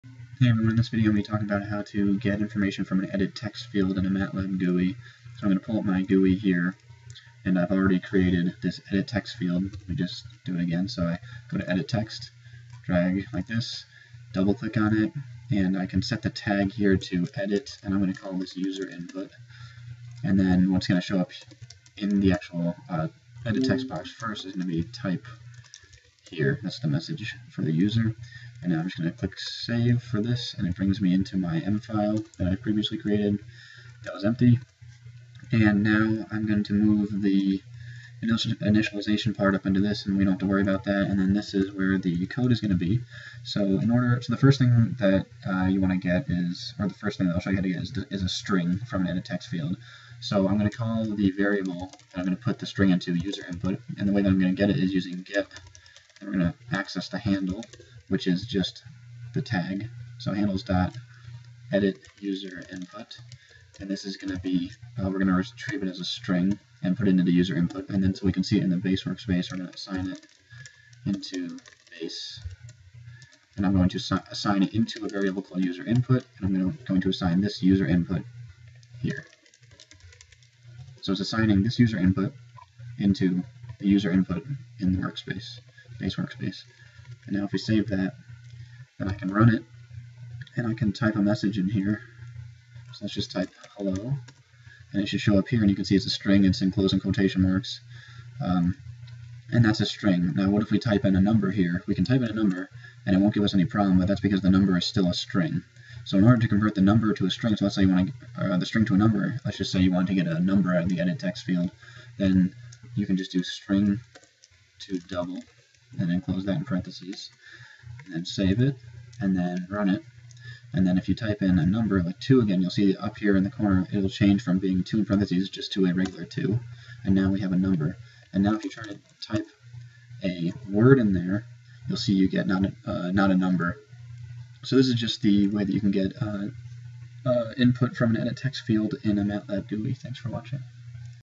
Explained: Edit Text Box GUI [MATLAB]
Edit text boxes can be used to input numbers or strings in your GUI.  This video explains how create an edit text box and retrieve both string data and double data from it.